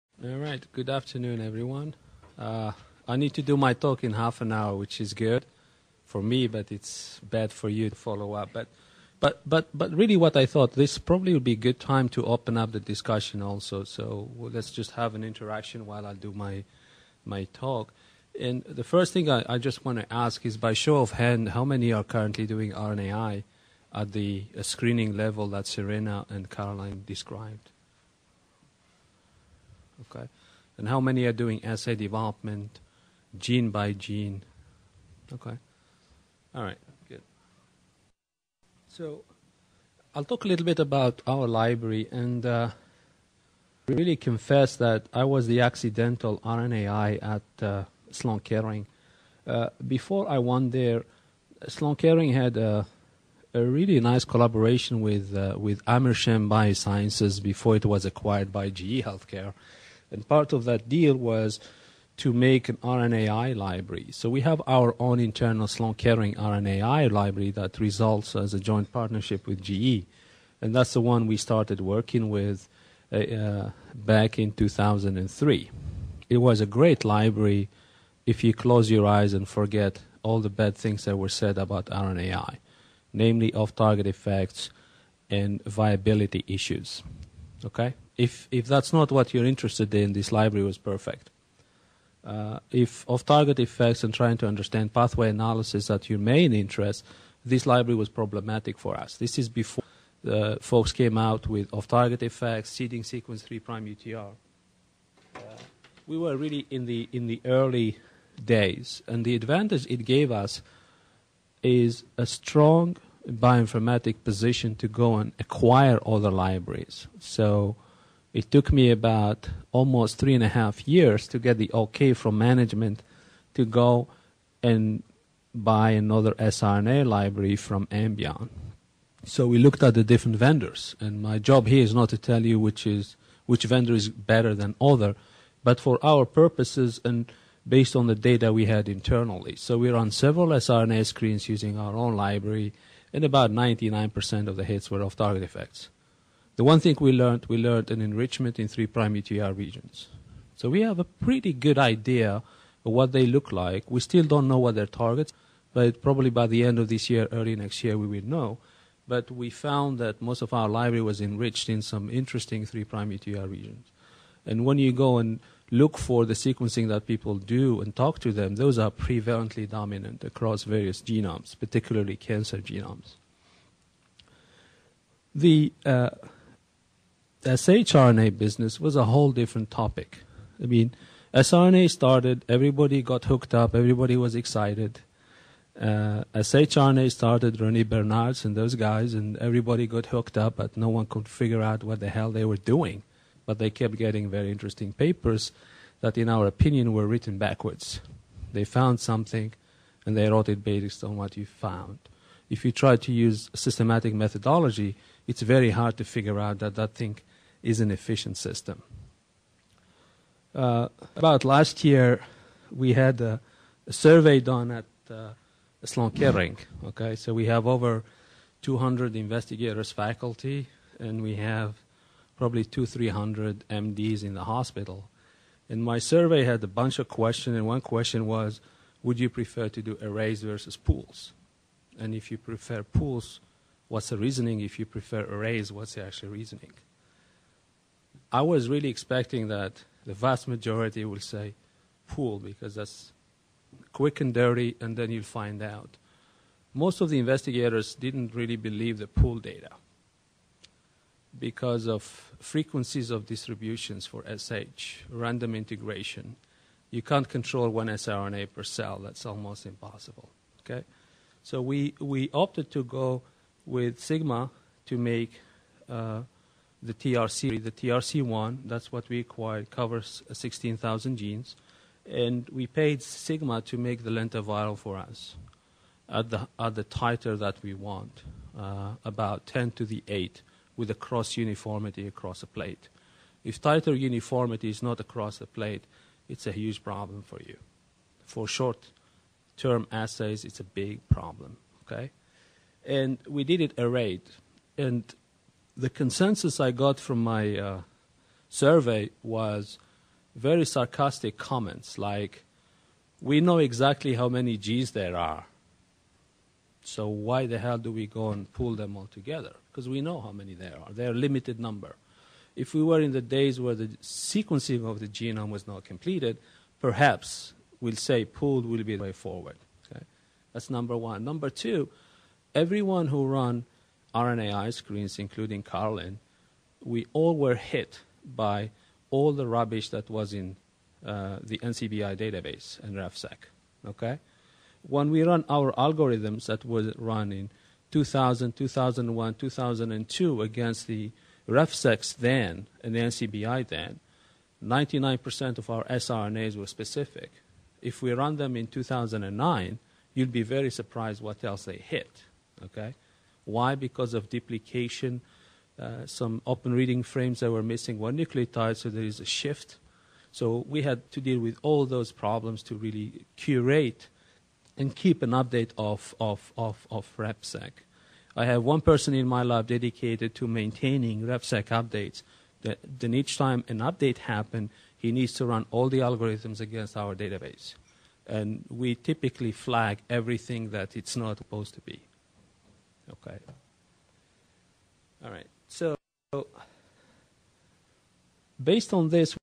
Strategies for Effective RNAi Screens
This course is designed to provide in-depth information on how to go about setting up RNAi screening experiments and how to design assays for getting optimal results. The challenges working with different types of molecules i.e. siRNAs, shRNAs and the delivery systems to get them into the appropriate cells and tissues will be discussed.